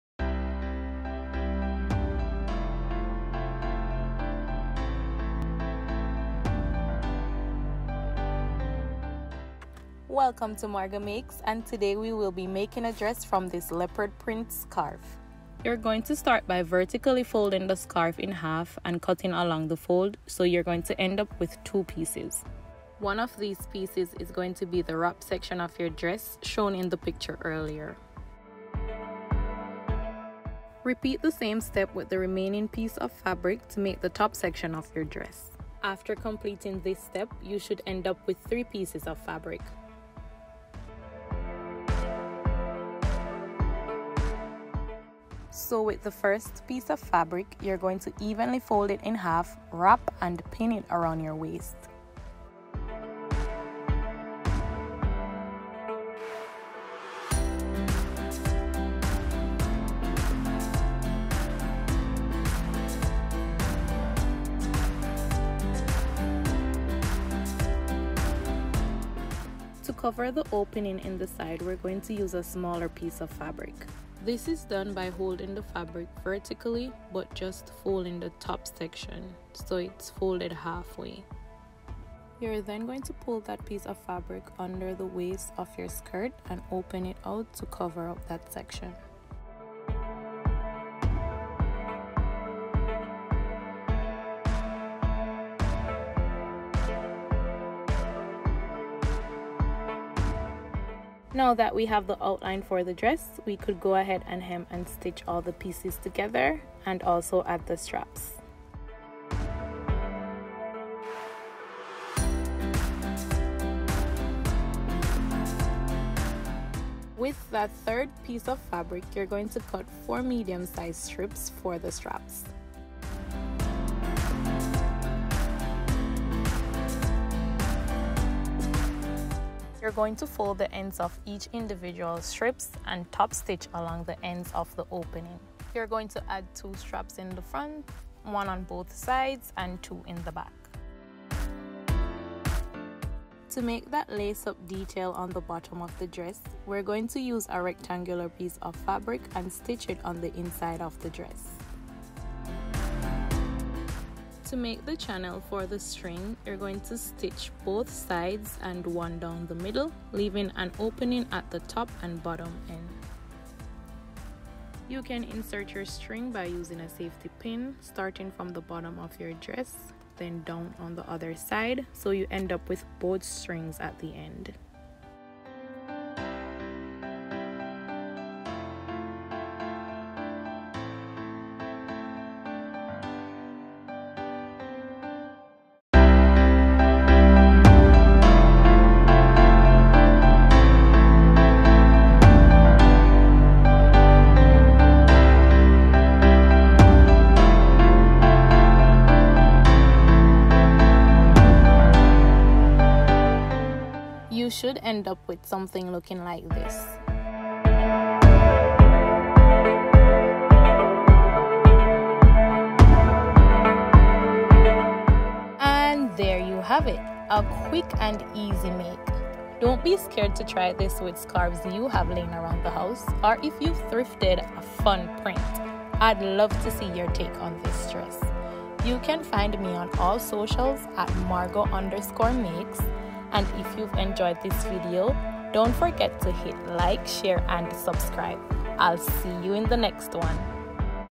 Leopard Print Scarf Upcycle Dress| DIY Fall Dress
Found this lovely scarf on sale at Zara for $3.99 and I was inspired to make a dress for Fall! It’s a quick and easy way to repurpose your old or thrifted scarfs. Hope you enjoy this video, tag me if you recreate it! I would love to see your version 🙃